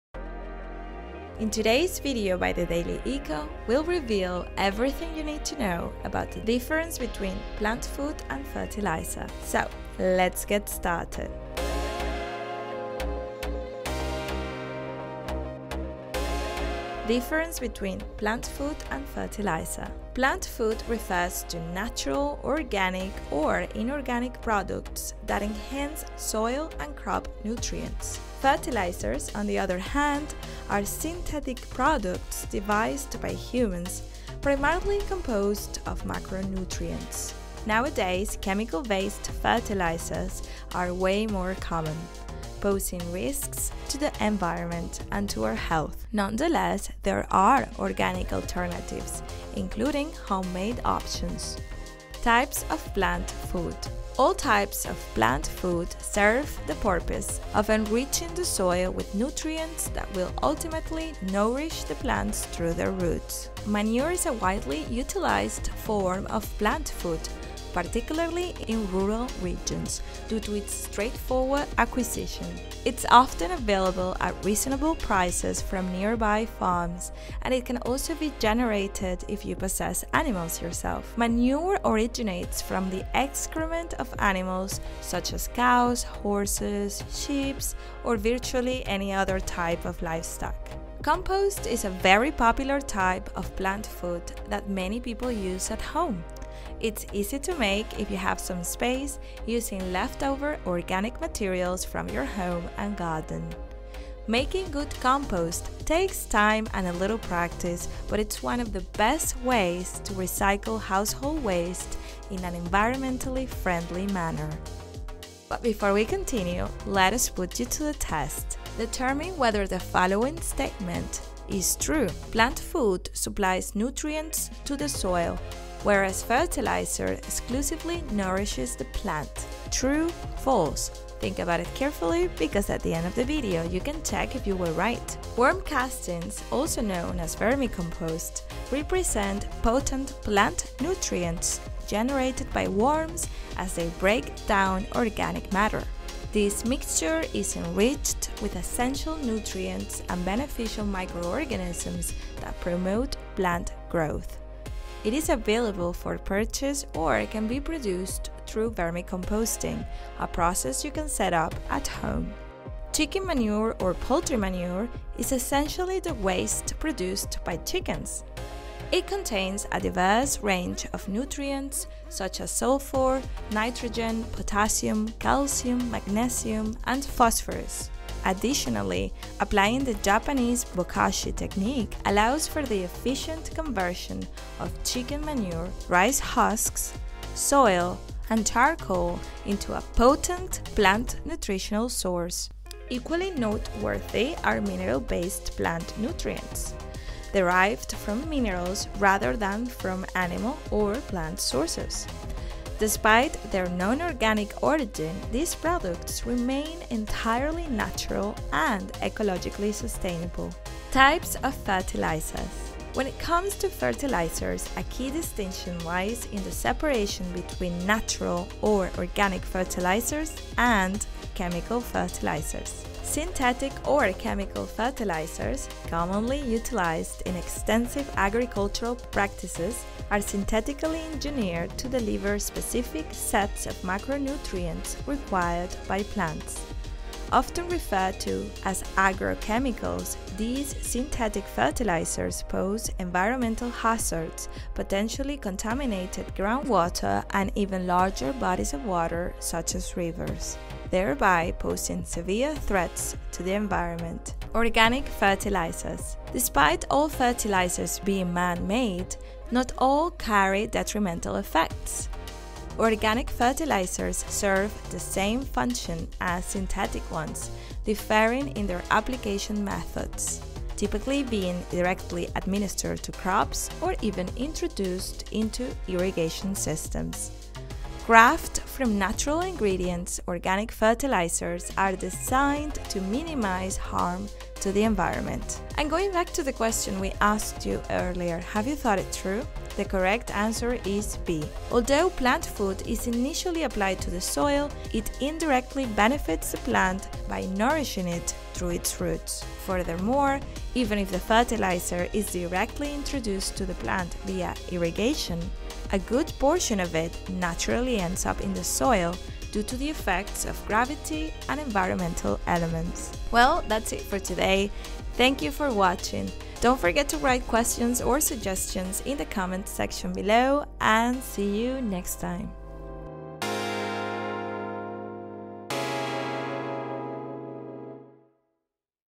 PLANT FOOD VS. FERTILIZER 🌱 What's the Difference?
🌱 Are you under the impression that PLANT FOOD and FERTILIZER are one and the same? It's a surprisingly common misconception. That's precisely why, in this informative video by thedailyECO, we're here to clarify the crucial DISTINCTION between PLANT FOOD and FERTILIZER.  Additionally, we'll highlight the contrasts between synthetic fertilizers and organic alternatives.

🌏 INDEX 🌏

00:00 Understanding the Distinction: Plant Food vs. Fertilizer
00:16 Exploring the Differences: Plant Food and Fertilizer
00:53 Exploring Types of Plant Food
03:12 Examining Various Categories of Fertilizers